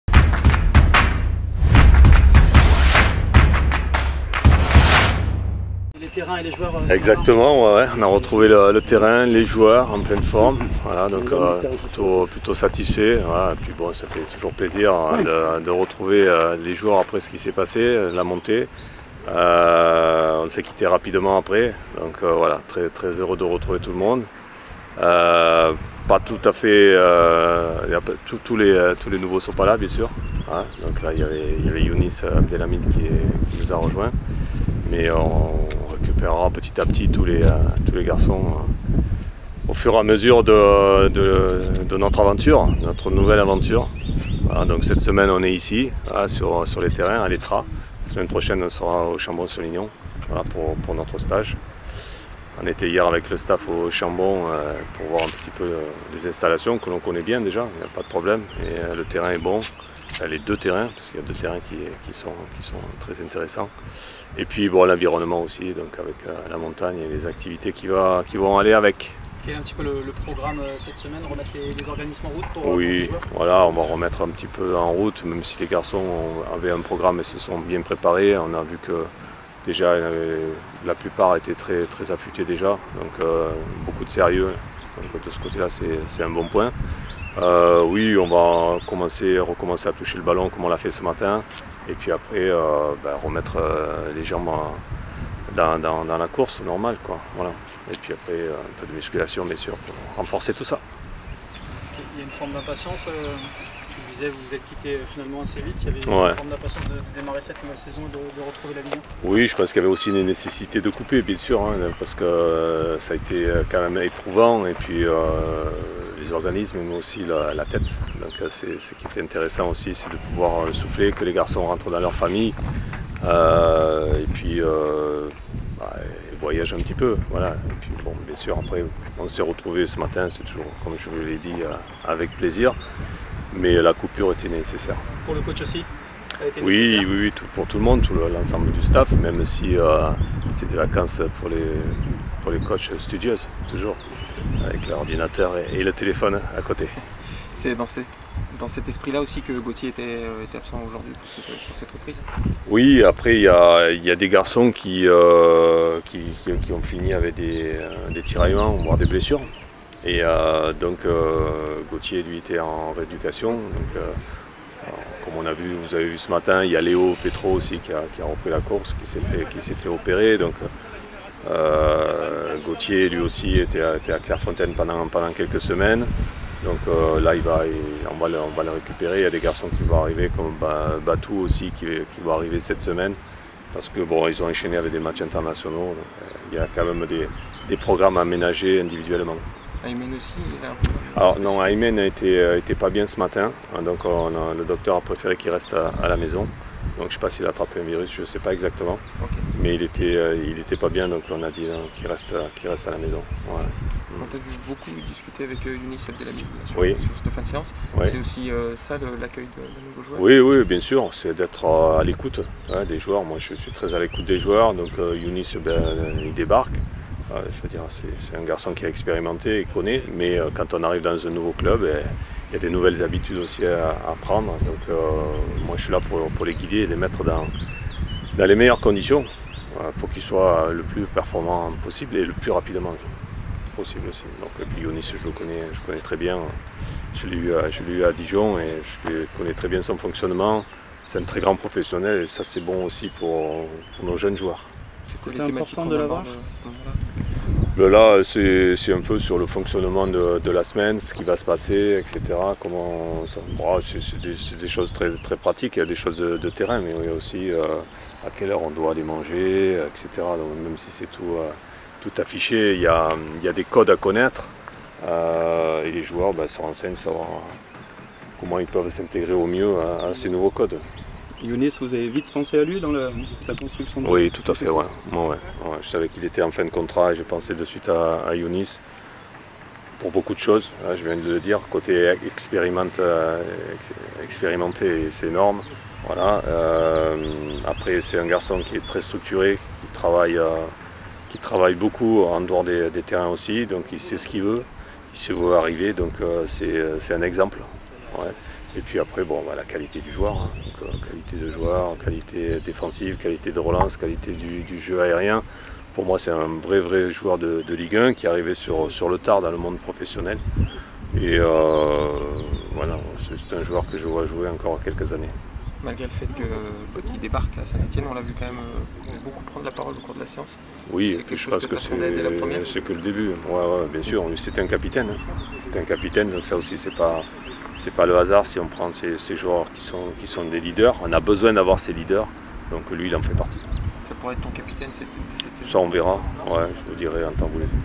Dall'Oglio : "Tous les nouveaux ne sont pas encore là"
À l'issue de séance de reprise ce matin pour l'AS Saint-Étienne, le coach stéphanois, Olivier Dall'Oglio, s'est exprimé à notre micro durant quelques minutes. Il évoque bien évidemment ce retour à L'Etrat et semble confier à demi-mots que plusieurs renforts devraient débarquer dans le Forez incessamment sous peu.